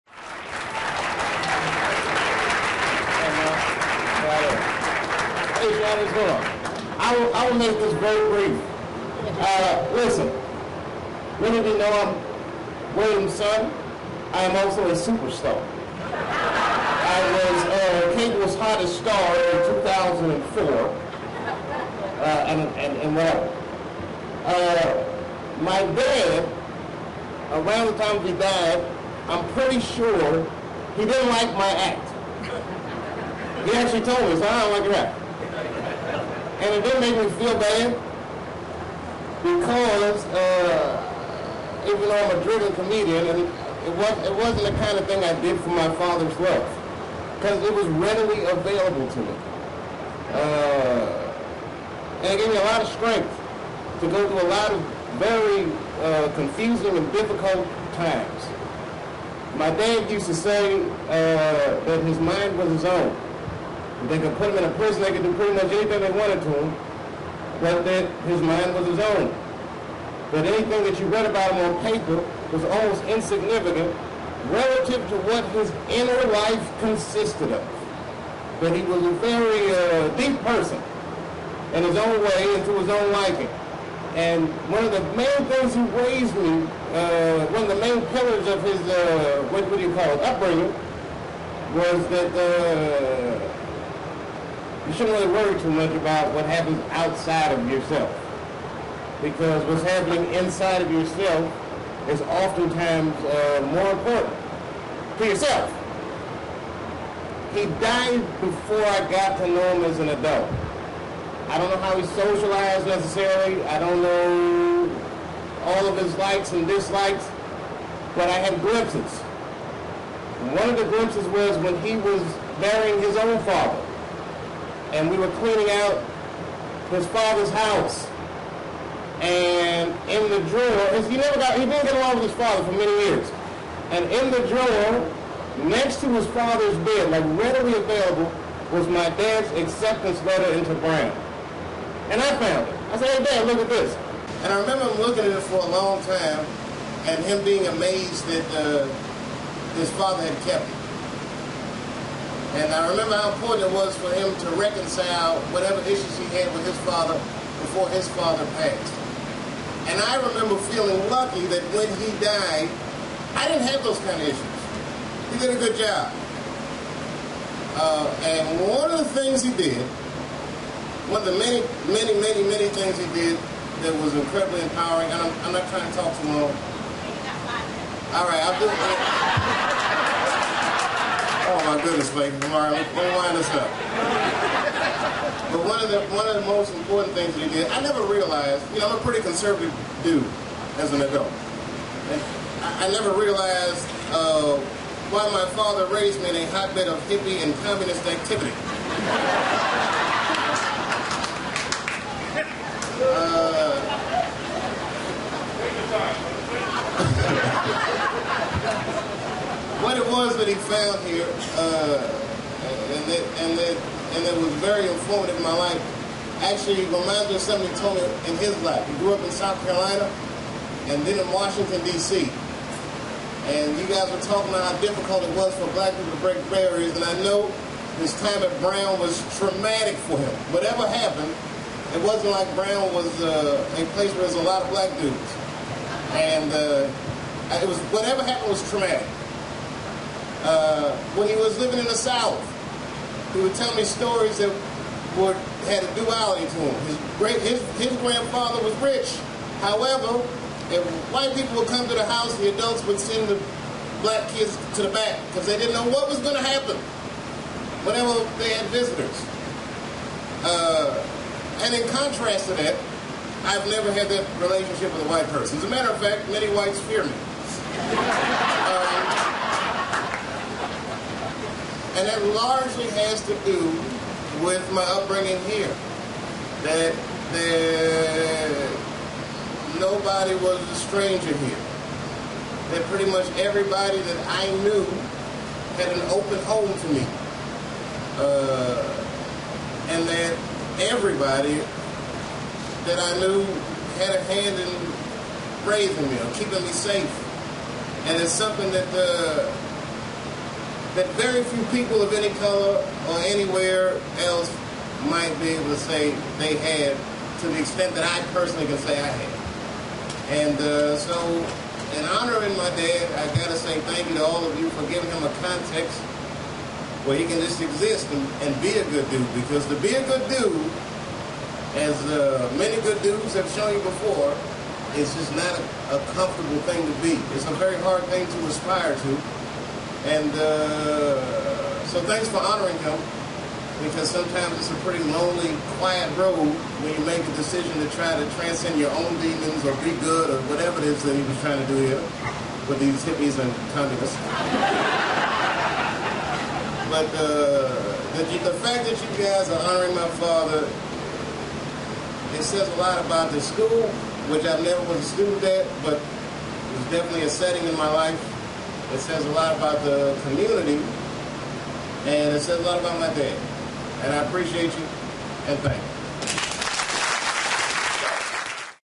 Comedian David Chappelle - closing remarks for his late father William David Chappelle III
Comedian David Chappelle's closing remarks for his late father William David Chappelle III an Walter F. Anderson Award recipient. His father served as a Co-op Department faculty member, dean of community services and adjunct professor of music.